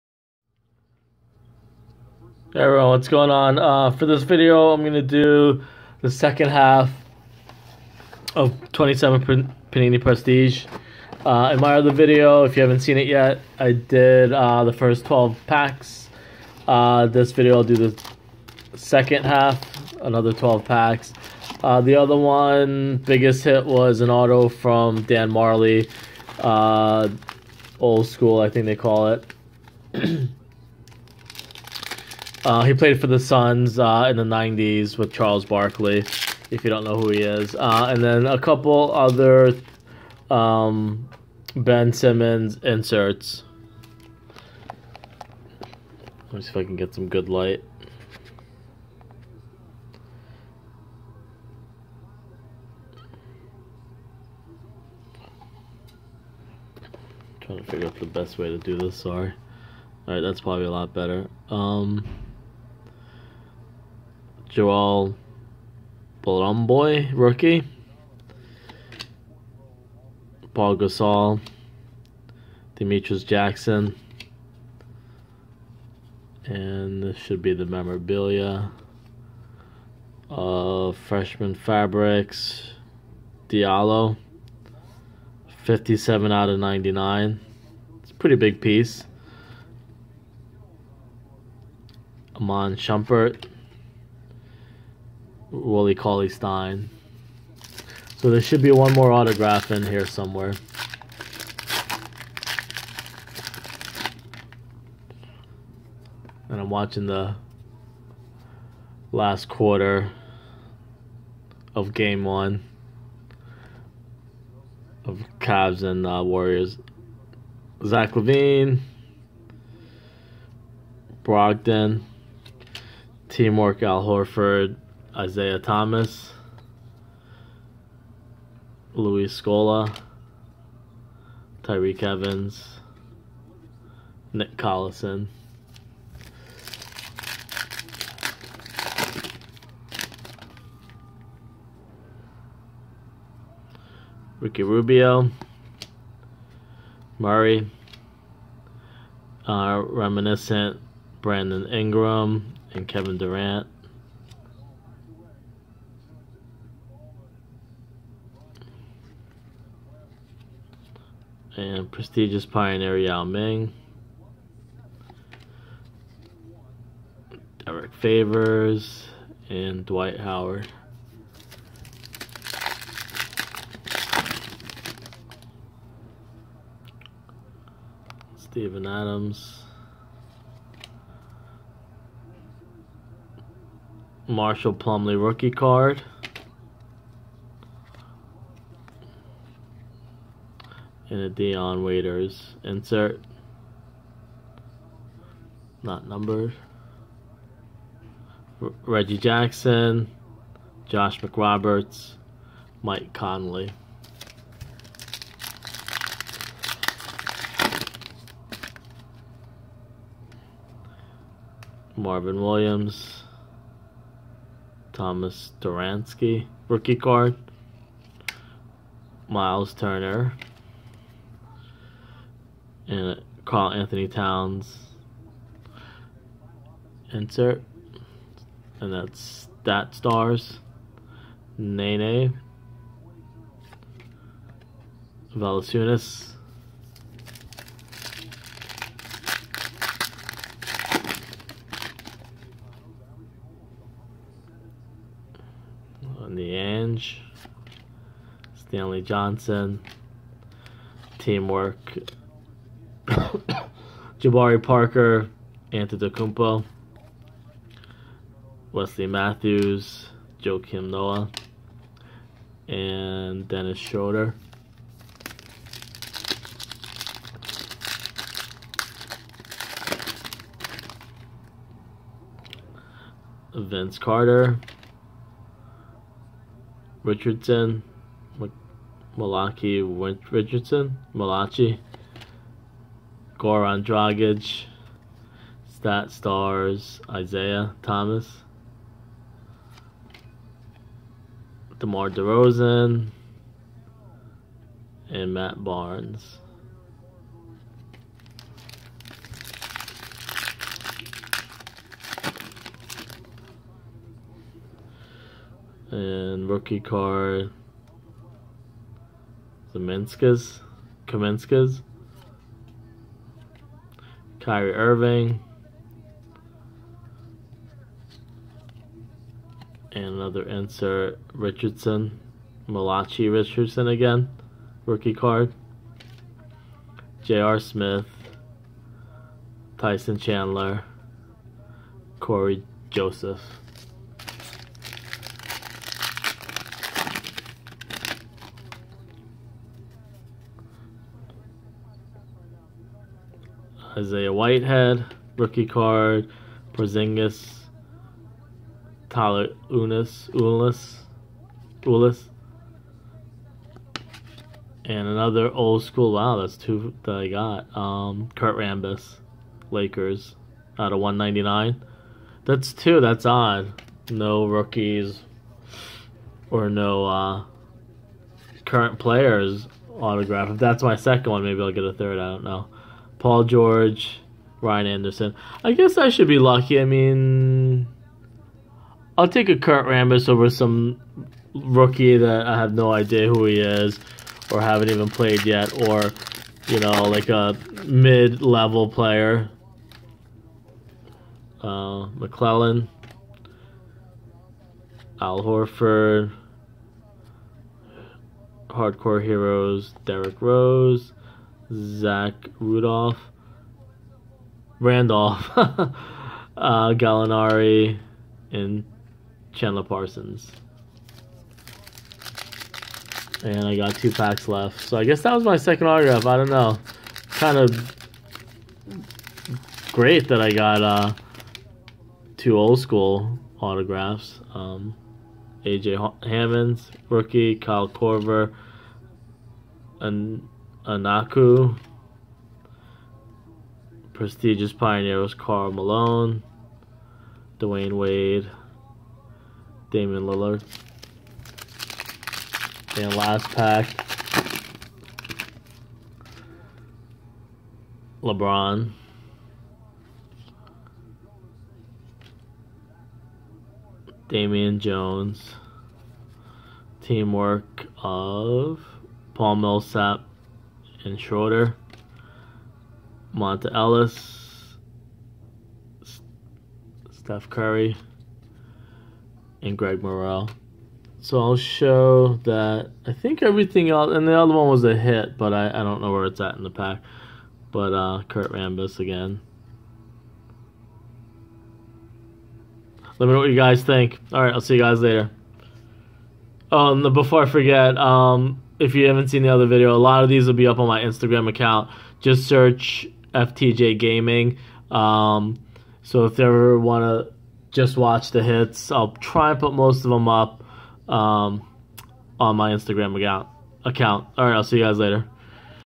2017 Panini NBA Prestige hobby box opening video #2. Kurt Rambis autograph.
video #2. Cards will be uploaded on my Instagram